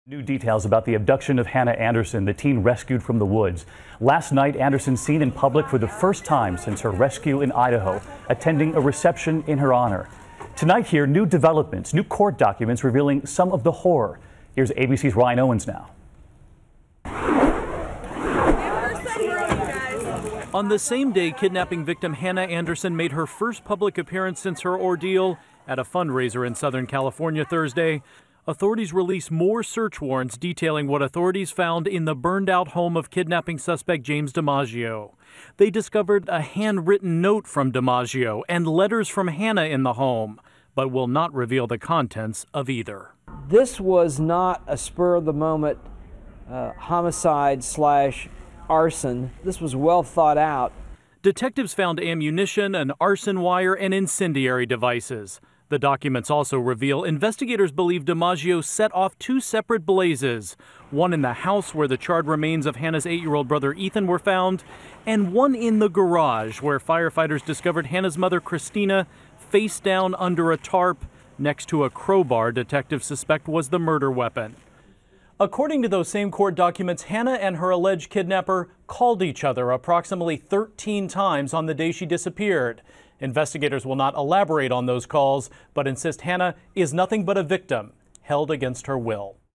Hannah Anderson's First Public Appearance After Rescue
Rescued Teenager attends fundraiser after kidnapping ordeal.